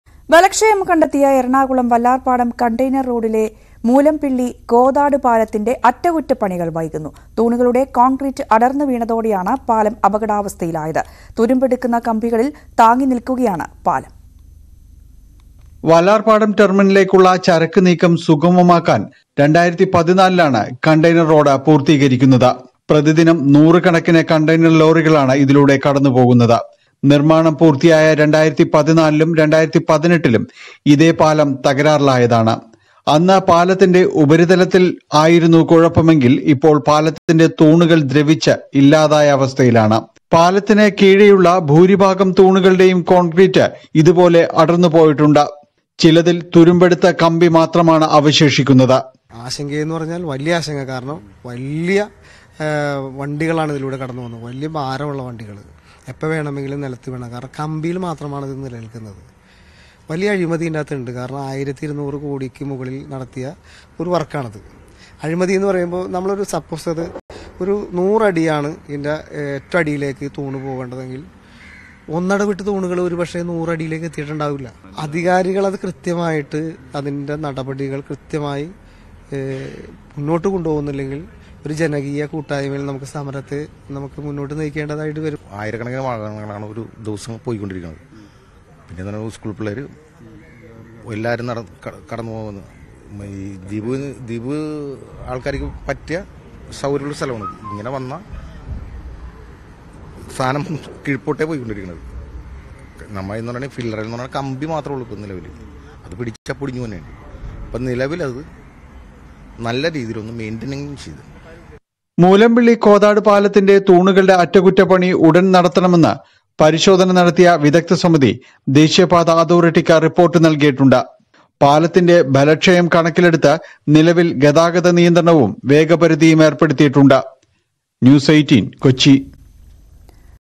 Ernakulam Vallarpadam Container Road അപകടാവസ്ഥയിൽ; പാലത്തിന്റെ കോൺക്രീറ്റ് അടർന്നു വീണു
ബലക്ഷയം കണ്ടെത്തിയ Ernakulam Vallarpadam Container Roadലെ മൂലമ്പിള്ളി–കോതാട് പാലത്തിന്റെ അറ്റകുറ്റപ്പണികൾ വൈകുന്നു. തൂണുകളുടെ  കോൺക്രീറ്റ് അടർന്നു വീണതോടെയാണ് പാലം അപകടാവസ്ഥയിലായത്.